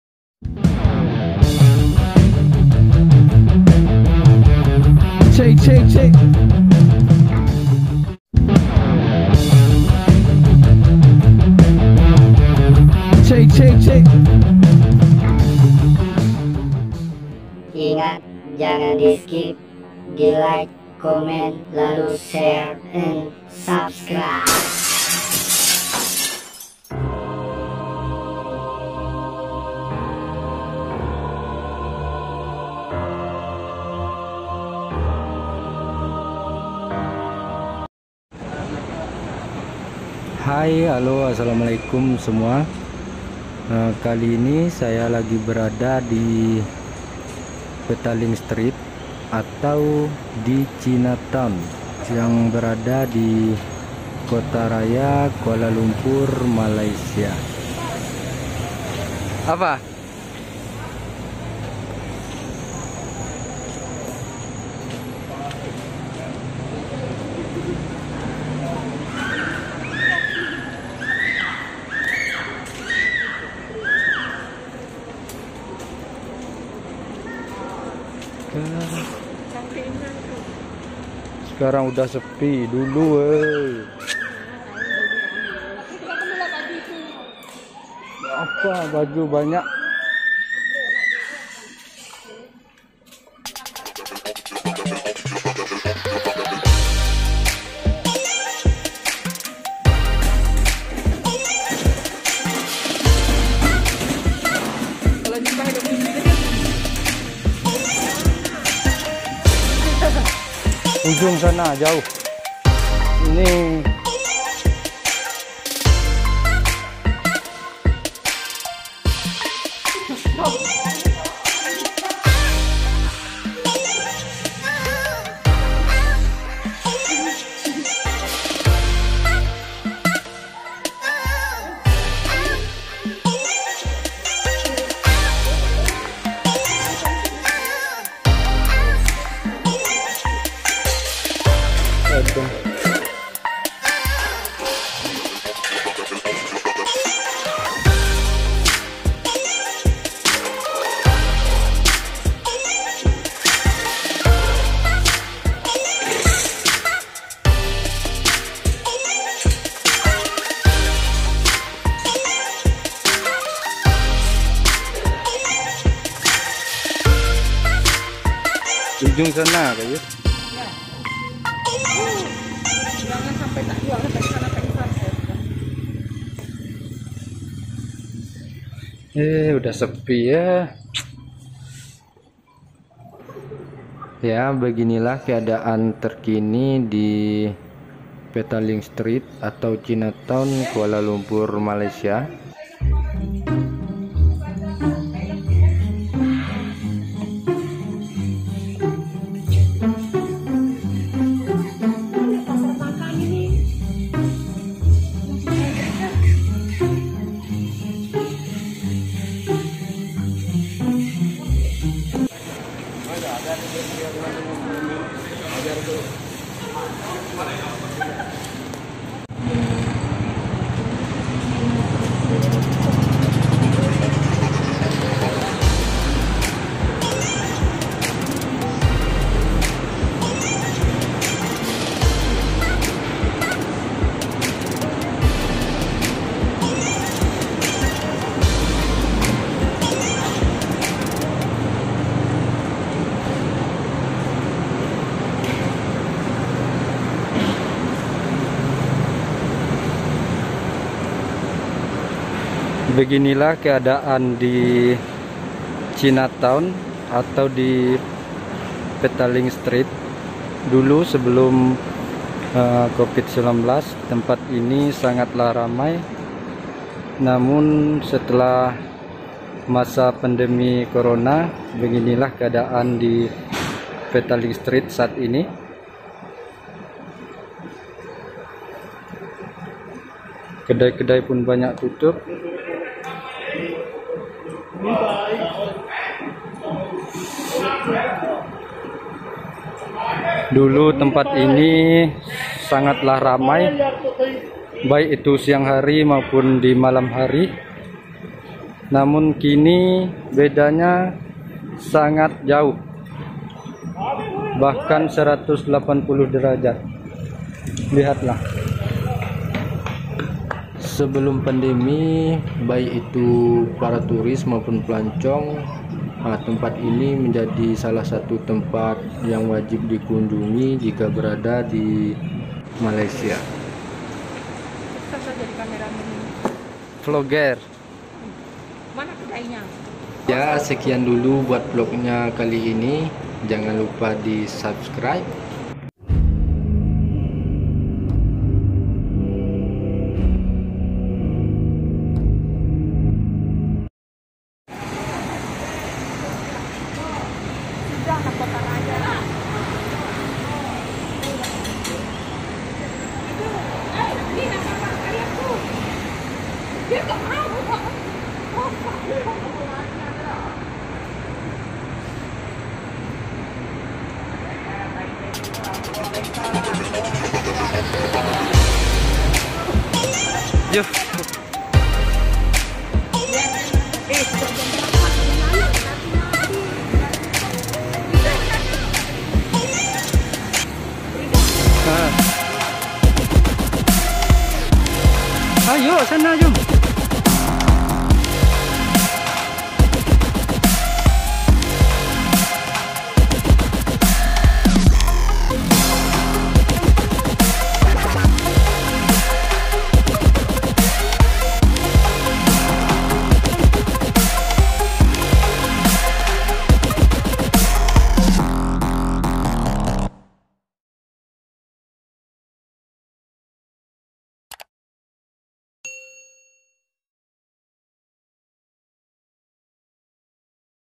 PETALING STREET CHINA TOWN KOTA RAYA KUALA LUMPUR MALAYSIA
Petaling Street China Town Kuala Lumpur Malaysia
Suasana terkini Petaling Street 2021 pasca pandemi Covid-19 China Town Jalan Petaling Kota Raya KL